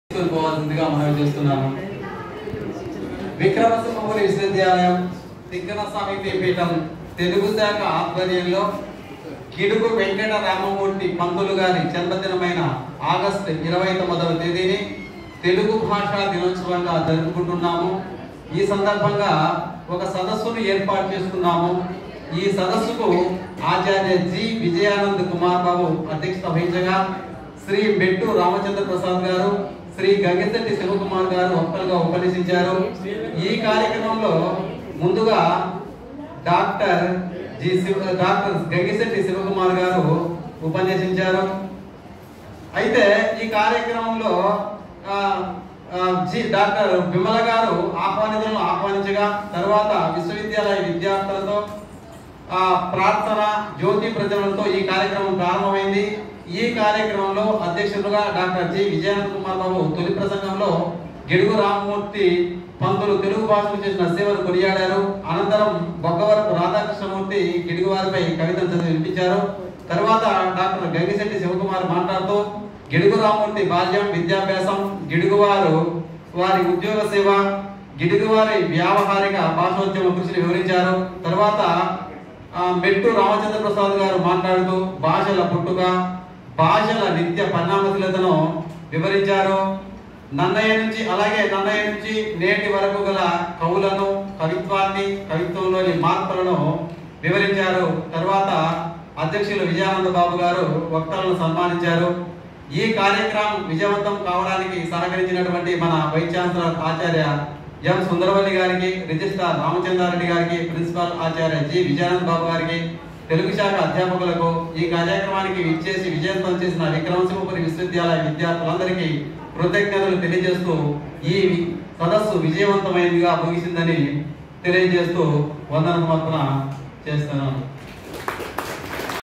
TELUGU LANGUAGE DAY VOTE OF THANKS SPEECH BY Dr.LAXMI NARAYANA REDDY IN VIKRAM SIMHAPURI UNIVERSITY
TELUGU LANGUAGE DAY VOTE OF THANKS SPEECH BY Dr.LAXMI NARAYANA REDDY IN VIKRAM SIMHAPURI UNIVERSITY NELLORE